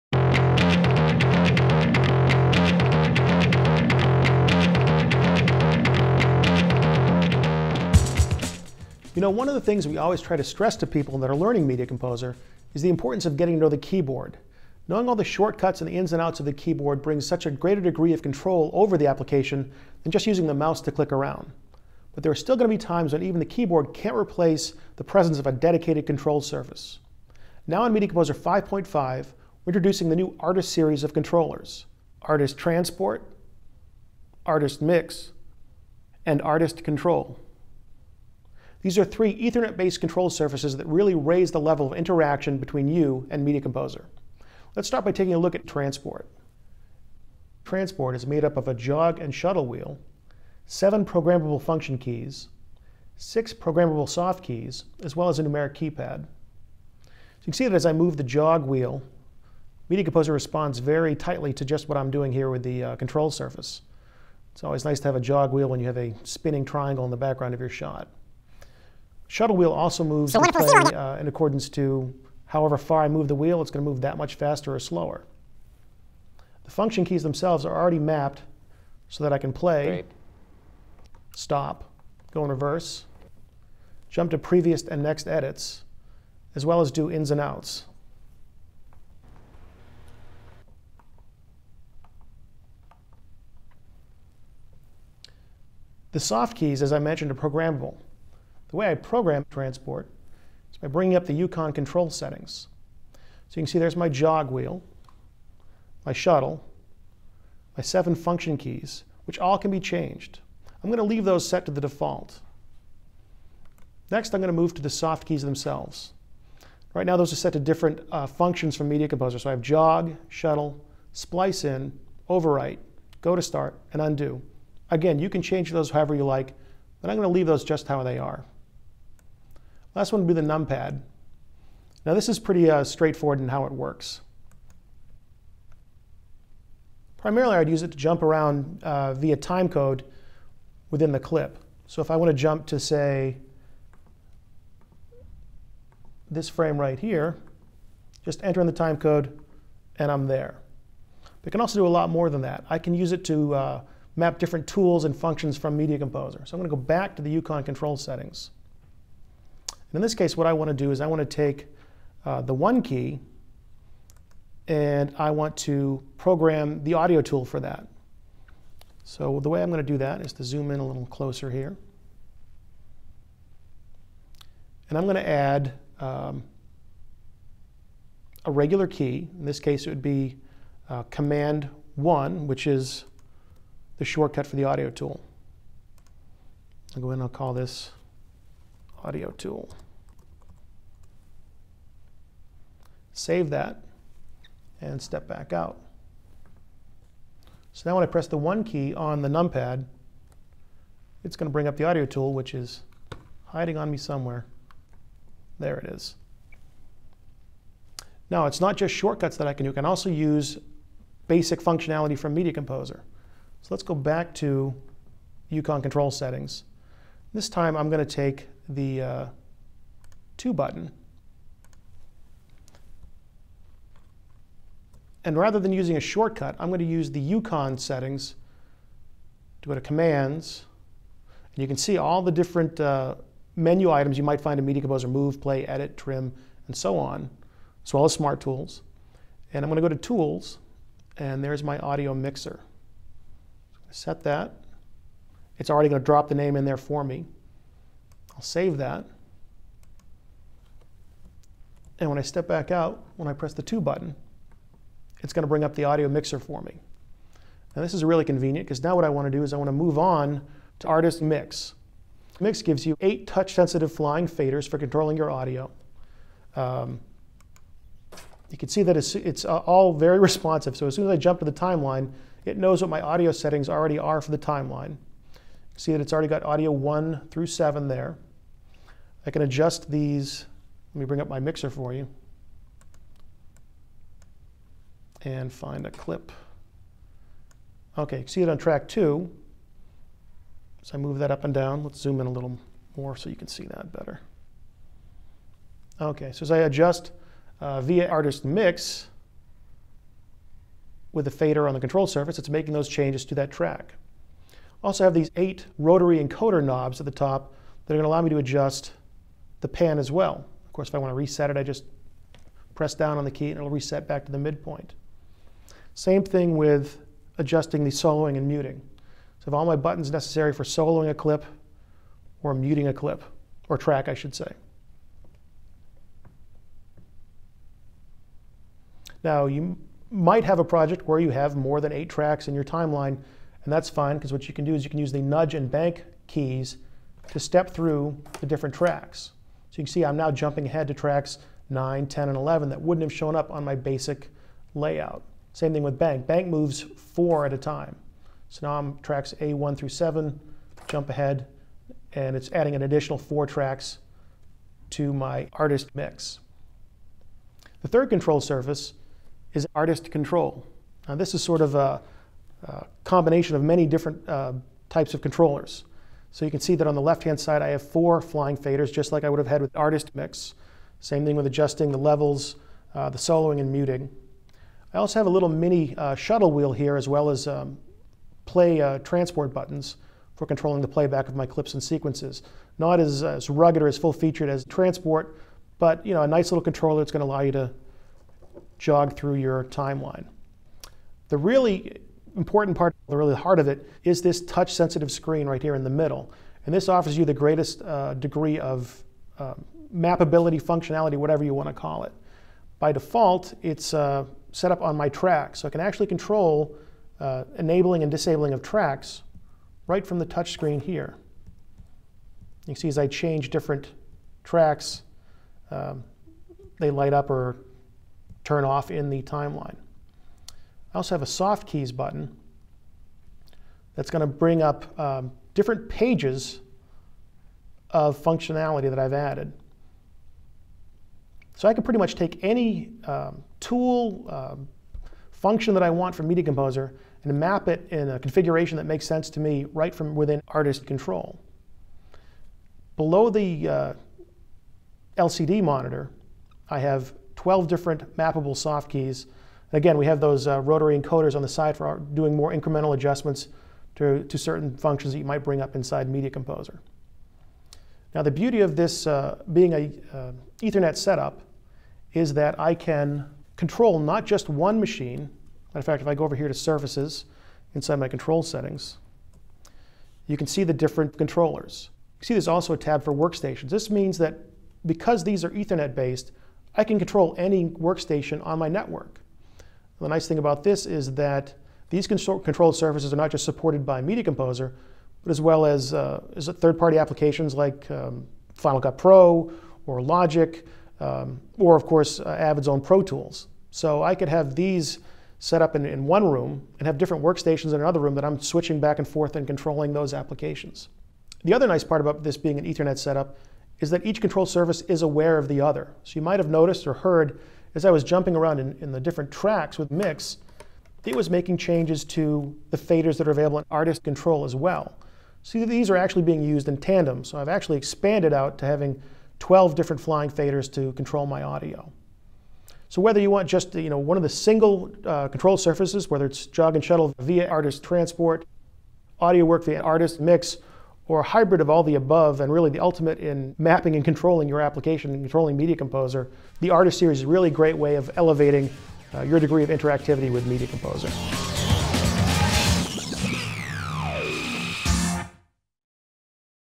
Media Composer® 5.5 ‒ Avid® Artist Controllers
One of the things we try to stress to people when they're getting to know Media Composer, is the importance of getting to know the keyboard. Knowing all the shortcuts and ins-and-outs of the keyboard brings a greater degree of control over the application than just using the mouse to click-around. But there are still going to be times when the keyboard cannot replace the presence of a dedicated control surface. Now in Media Composer 5.5 we're introducing the Artist Series of controllers—Artist Transport, Artist Mix, and Artist Control. These are three Ethernet-based control surfaces that really raise the level of interaction between you and Media Composer. Let's take a look.